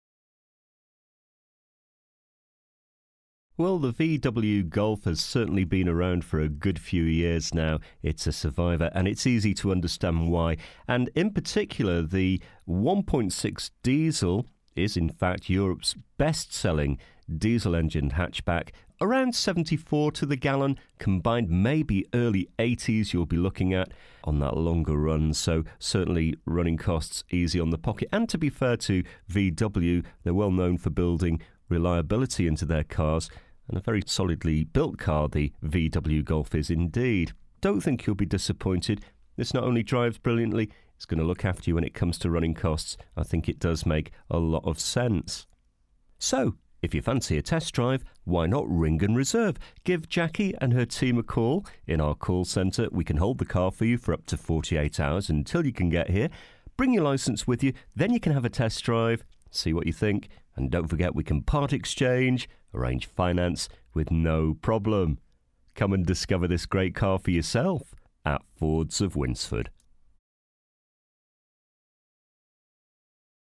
VOLKSWAGEN GOLF DIESEL HATCHBACK (2011) 1.6 TDI 105 MATCH 3DR - SA61OAX
DETAILS:
First registered: 16/09/2011
Current mileage: 15080
Registration: SA61OAX
Colour: WHITE

FEATURES:
front electric windows, remote central locking, cloth interior, all round airbags, air conditioning, pas, abs, alloy wheels, solid paint, cd-6 multichanger, traction control, parking control, parcel shelf present, hand book present, cruise control, bluetooth, DAB radi

This used car is for sale at Fords of Winsford Car Supermarket in Cheshire, UK